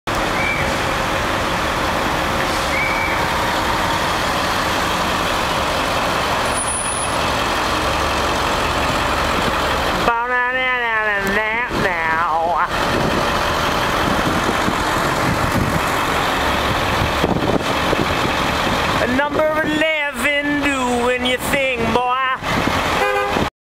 ANGRY EEL plaque #11
EEL #11 from the 1 thru 30 September 2009 series spotted on the streets of Brooklyn.